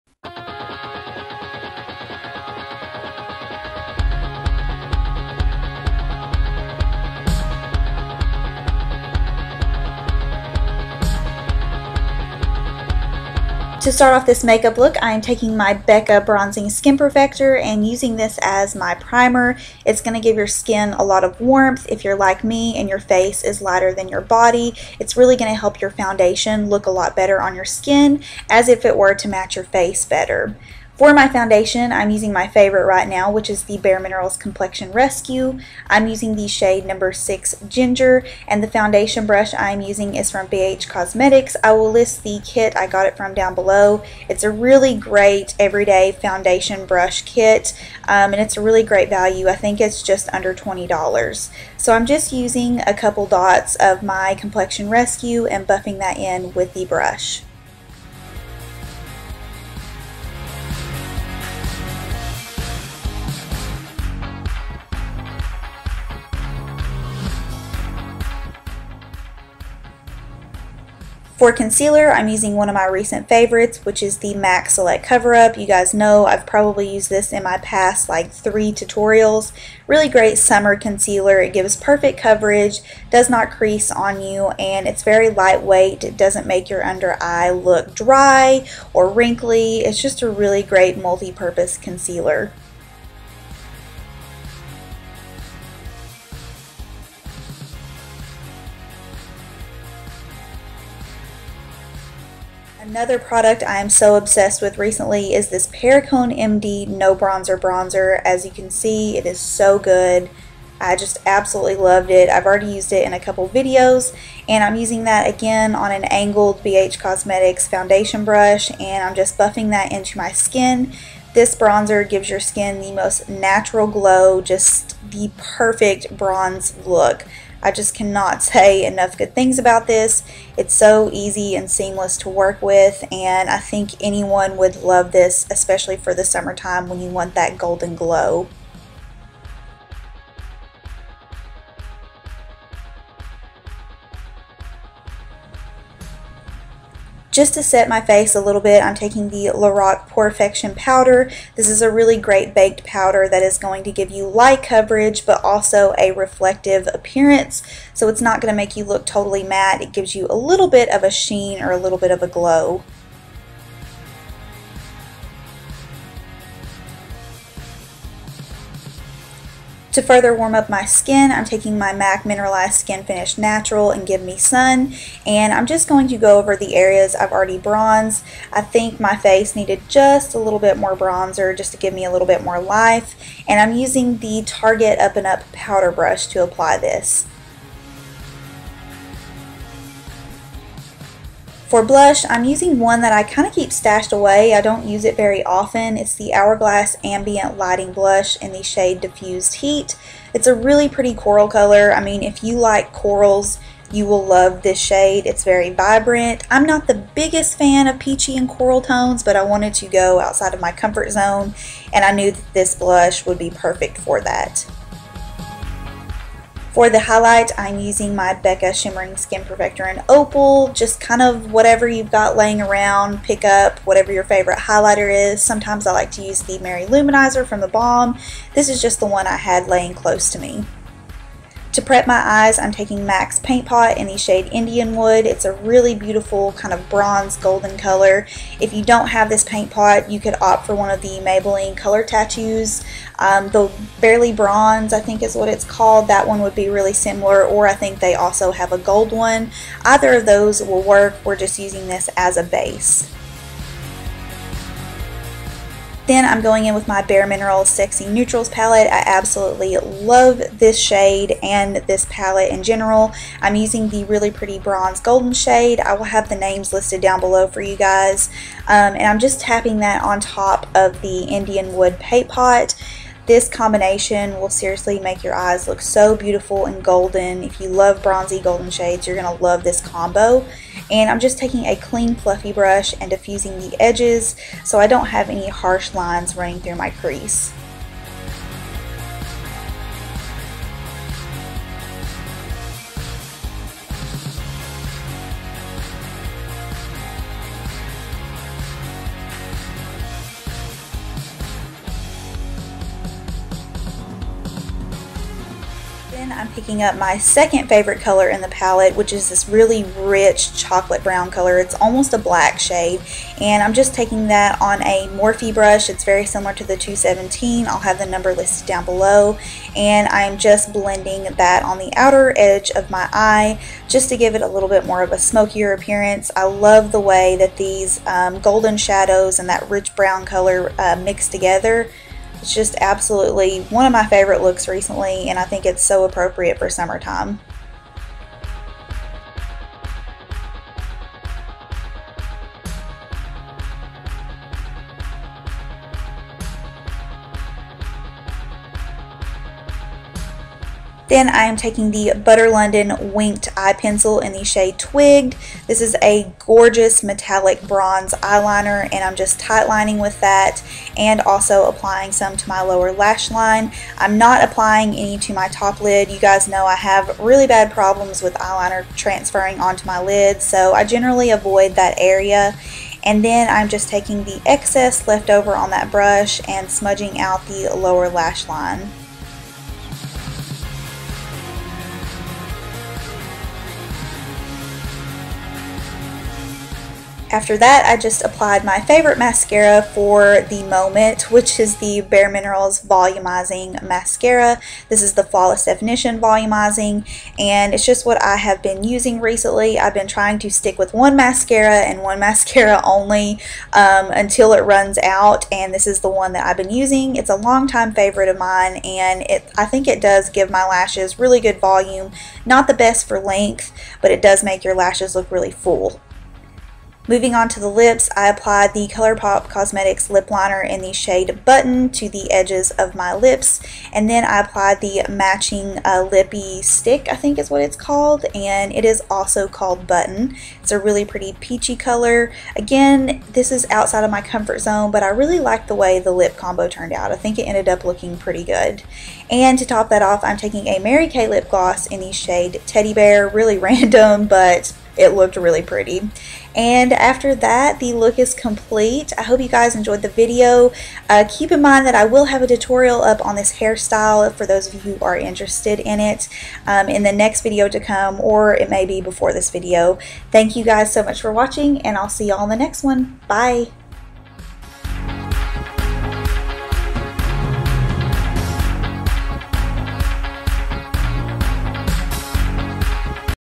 Bronzed Peach - Summer Makeup Tutorial | LoveShelbey
Products Mentioned:
BECCA Bronzing Skin Perfector
Bare Minerals Complexion Rescue (Ginger)
MAC Select Cover Up (NW25)
Perricone MD No Bronzer Bronzer
MAC Give Me Sun MSF
Hour Glass Ambient Lighting Blush (Diffused Heat)
Lorac Porefection Powder (Medium)
BECCA Shimmering Skin Perfector (Opal)
Anastasia Brow Wiz (Chocolate)

Eyes:
MAC Indian Wood Paint Pot
Bare Minerals Sexy Neutrals 8.0 Palette (Hot Commodity & Perspective)
Butter London "twigged" eye liner
Bare Minerals Volumizing Mascara

Lips:
Colour Pop "Button" Lippie Stix and Liner
Mary Kay @ Play "Teddy Bear" Gloss (Buxom "White Russian" is similar)